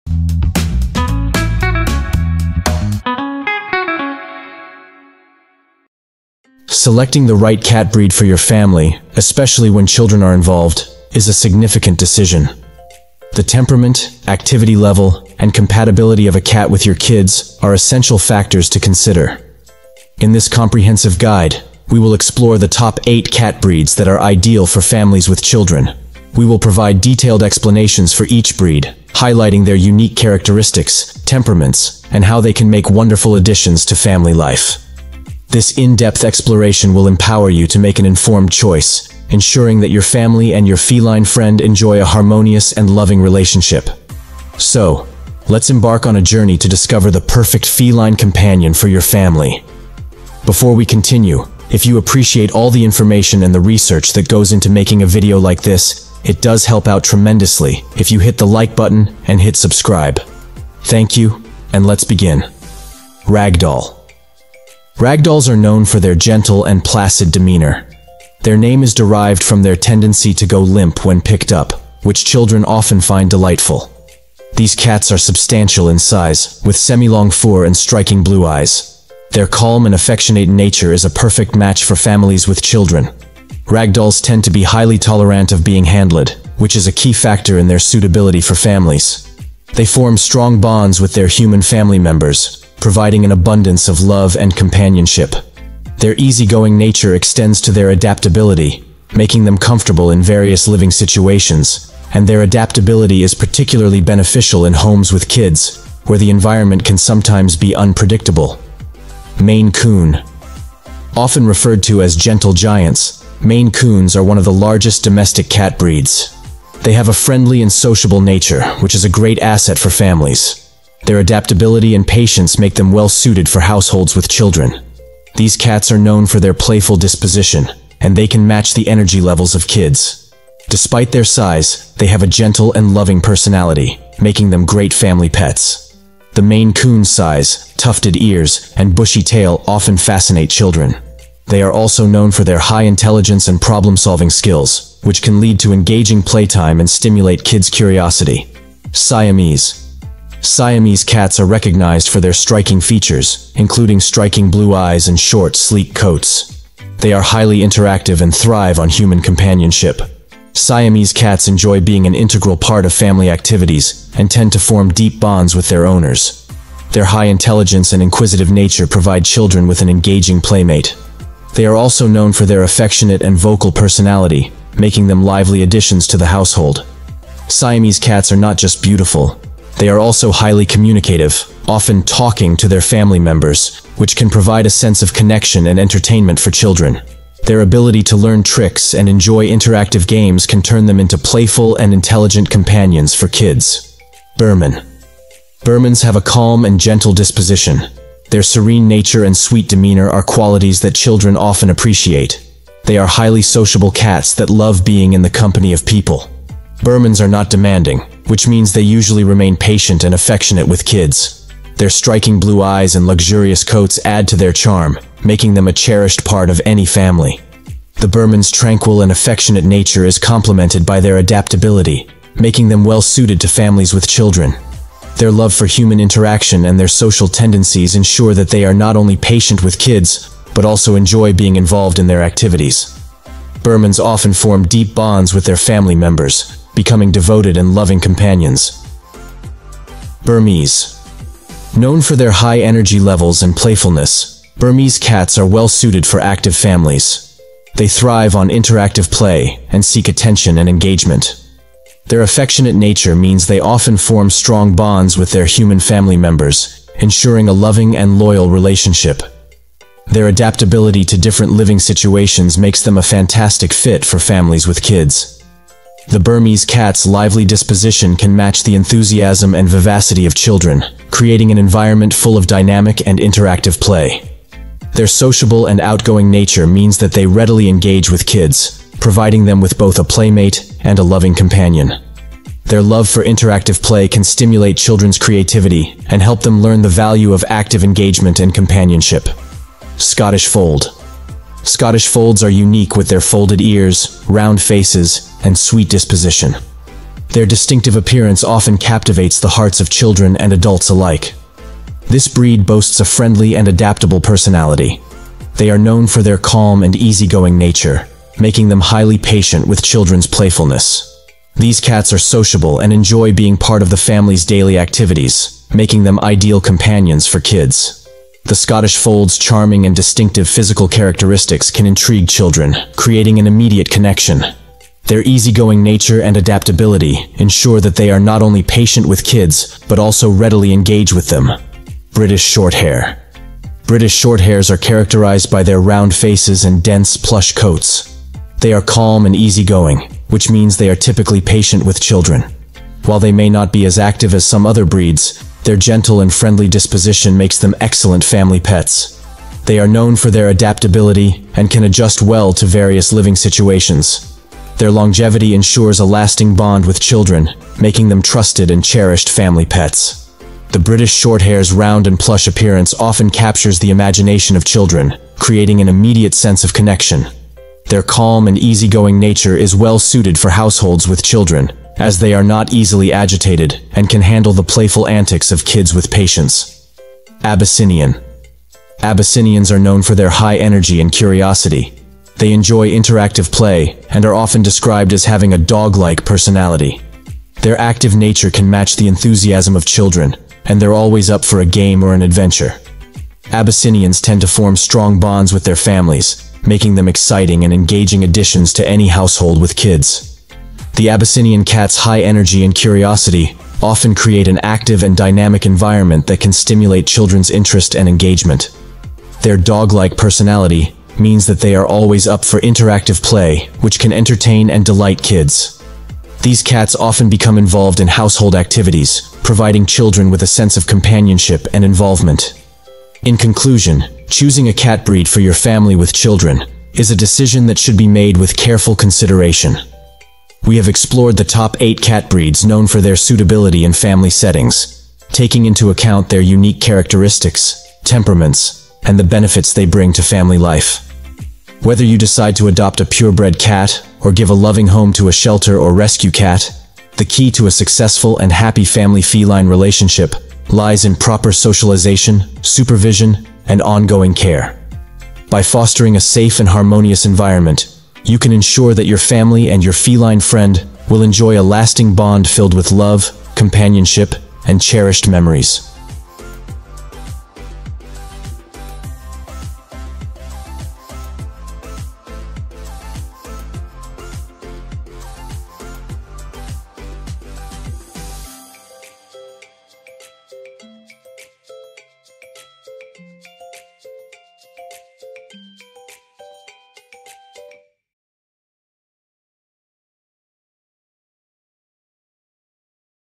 Top 8 Cat Breeds for Families with Children
Selecting the right cat breed for your family, especially when children are involved, is a significant decision. The temperament, activity level, and compatibility of a cat with your kids are essential factors to consider. In this comprehensive guide, we will explore the top 8 cat breeds that are ideal for families with children. We will provide detailed explanations for each breed, highlighting their unique characteristics, temperaments, and how they can make wonderful additions to family life. This in-depth exploration will empower you to make an informed choice, ensuring that your family and your feline friend enjoy a harmonious and loving relationship. So, let's embark on a journey to discover the perfect feline companion for your family!